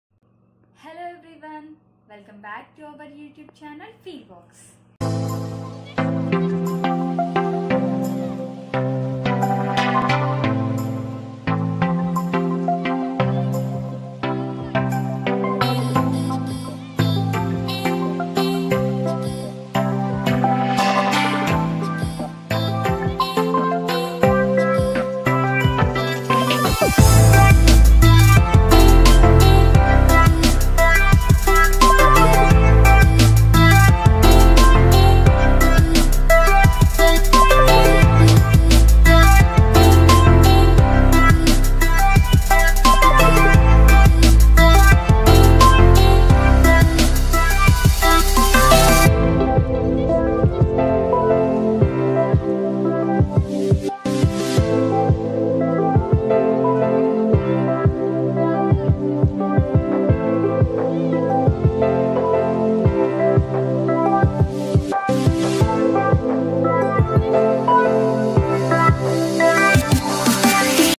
Resin momery stand | Resin Tabletop Handmade gifts by FeelBox Hyderabad | Gift for Bday Anniversary
Hello everyone.
Here we go presenting our RESIN MEMORY STAND
let us know your thoughts on our work by commenting below...

music credits.